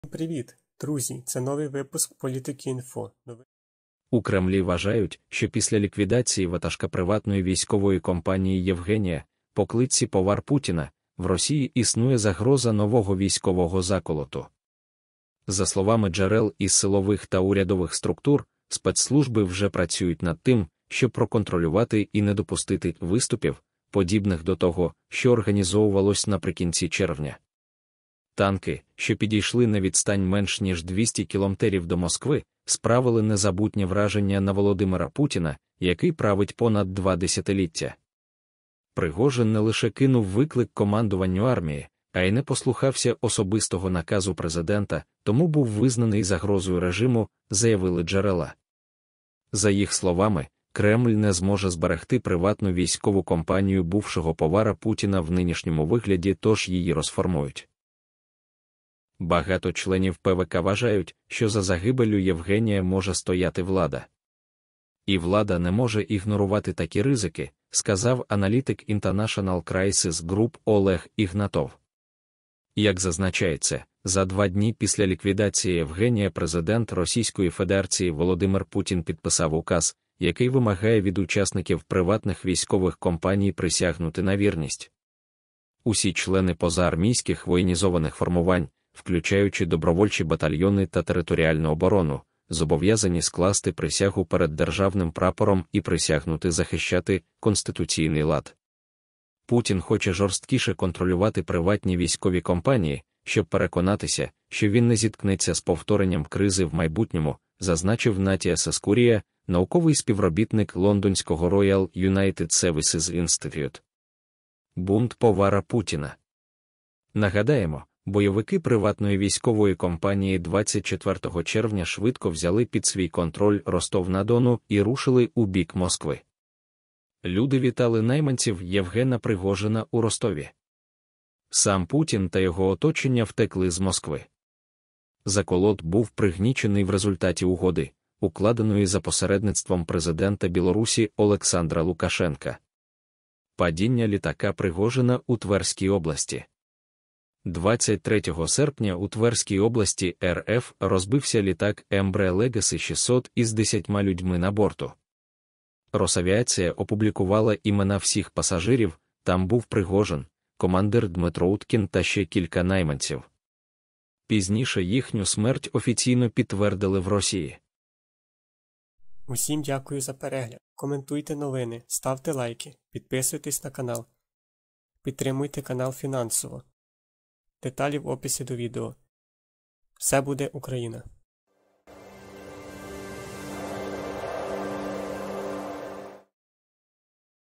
Кремль наказав спецслужбам готуватися до нової спроби перевороту, - ЗМІ
У Кремлі спостерігається загроза нового військового заколоту після ліквідації ватажка приватної військової компанії, відомого як "повар путіна". Спецслужби активно працюють над контролем над ситуацією та намагаються запобігти подібним "виступам". російський режим розглядає можливість розформування приватної військової компанії, щоб запобігти подальшим загрозам стабільності. Цей крок спрямований на жорсткий контроль над приватними військовими компаніями для запобігання кризам та неврегульованим діям.